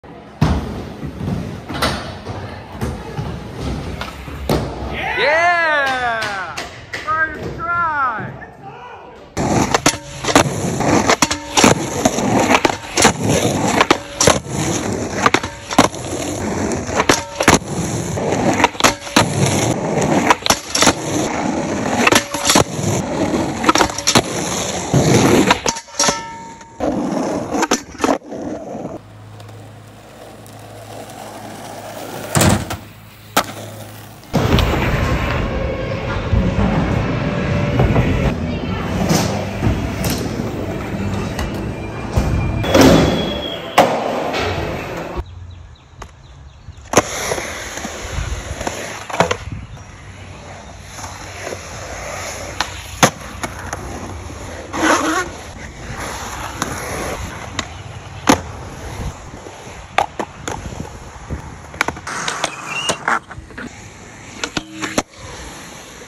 Cody Osten 2023
My last skate video was in 2015, I haven't been able to fully get back into it since, I was getting close in 2023, here's the video from that

Skateparks
Vessel Skatepark, Worcester Massachusetts
My home
The Edge, Taunton Massachusetts
Willimantic Skatepark, Willimantic Connecticut
Greenfield Skatepark, Greenfield Massachusetts